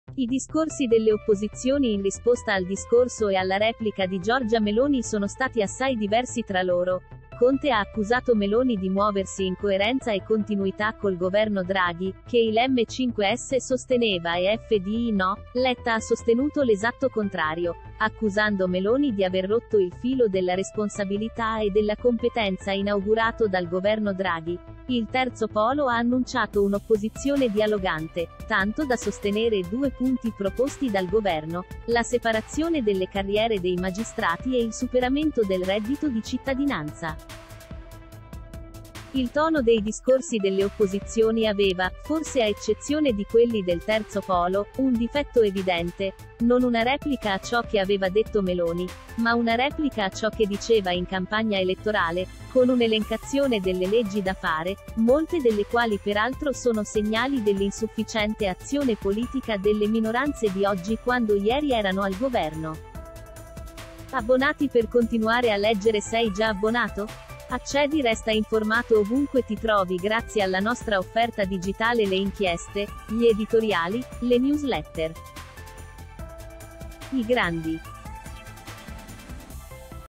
Le opposizioni messe in crisi da Meloni
I discorsi delle opposizioni in risposta al discorso e alla replica di Giorgia Meloni sono stati assai diversi tra loro: Conte ha accusato Meloni di muoversi in coerenza e continuità col governo Draghi (che  il M5s sosteneva e FdI no); Letta ha sostenuto l’esatto contrario, accusando Meloni di aver rotto il filo della responsabilità e della competenza inaugurato dal governo Draghi; il Terzo polo ha annunciato un’opposizione dialogante, tanto da sostenere due punti proposti dal governo, la separ